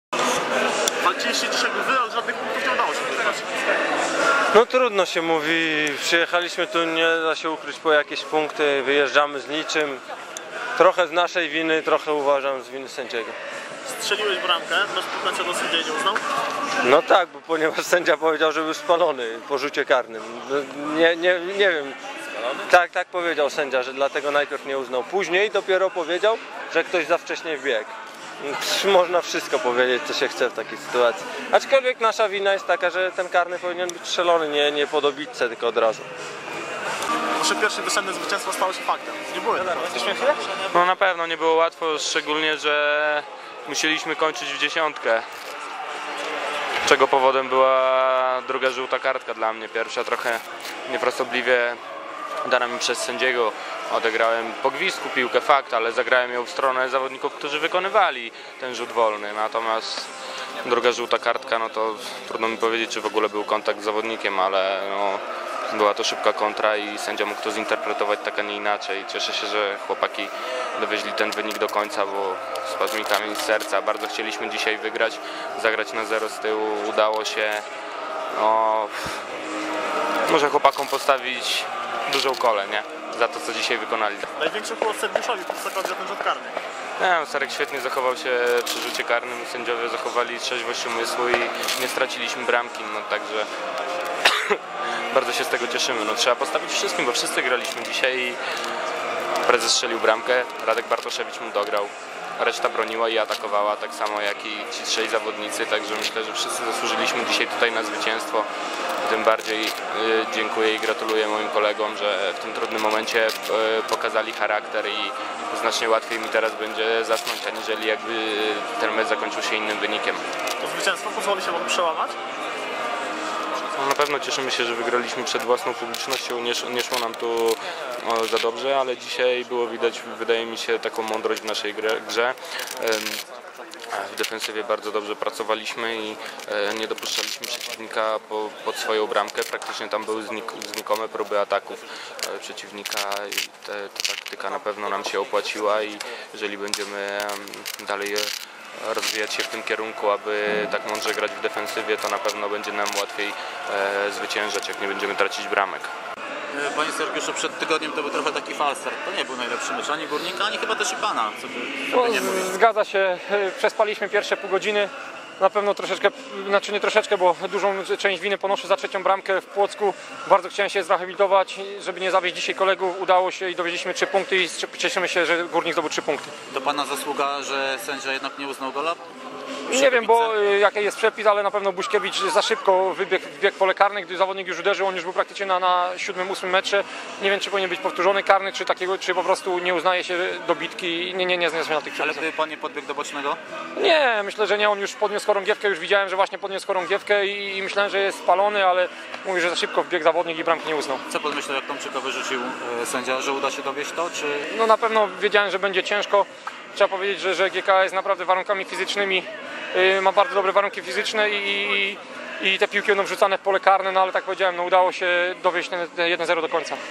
Wypowiedzi piłkarzy po meczu Górnik - GKS
Górnik Łęczna po zaciętym i pełnym kontrowersji spotkaniu pokonał GKS Katowice 1:0. Prezentujemy wypowiedzi na temat meczu piłkarzy obu zespołów.